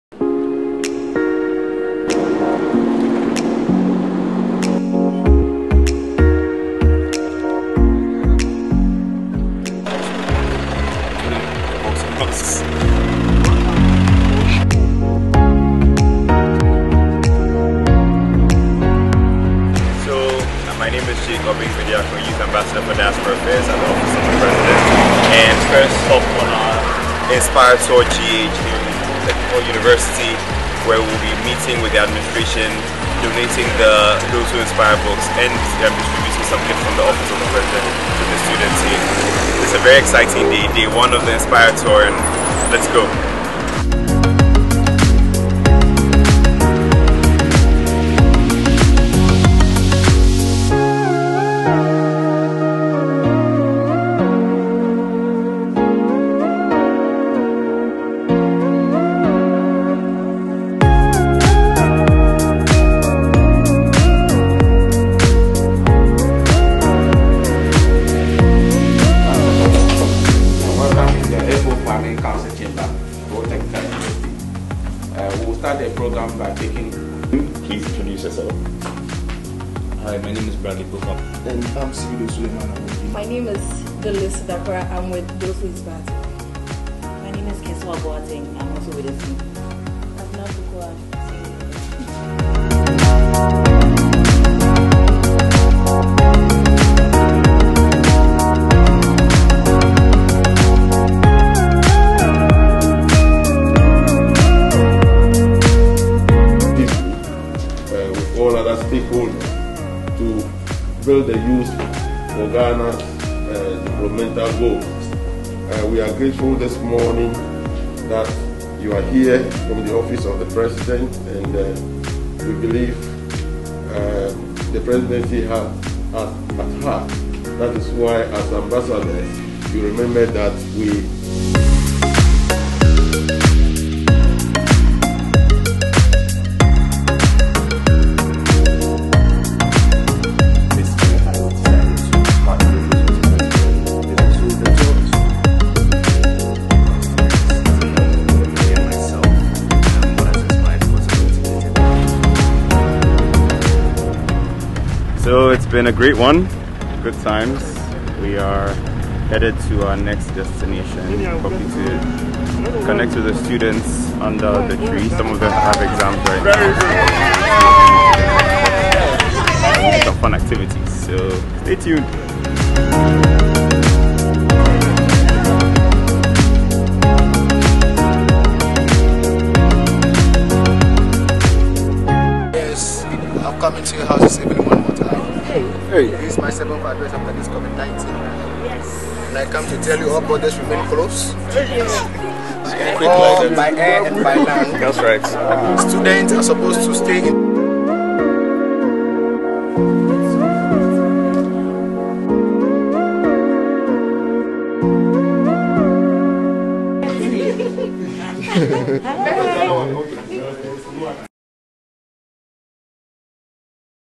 TWI Ghana: Inspiring Days - Day 1 Ho Technical University
“Those Who Inspire GHANA” is the 10th book of TWI bookcollection. We are delighted and honoured to start the inspiring days tour in Ghana in collaboration the Office of the President @nakufoaddo.
Follow us and @inspiretourgh and discover more about thosewhoinspiremovement in ghana🇬🇭.
Special thanks to Jake Bediako.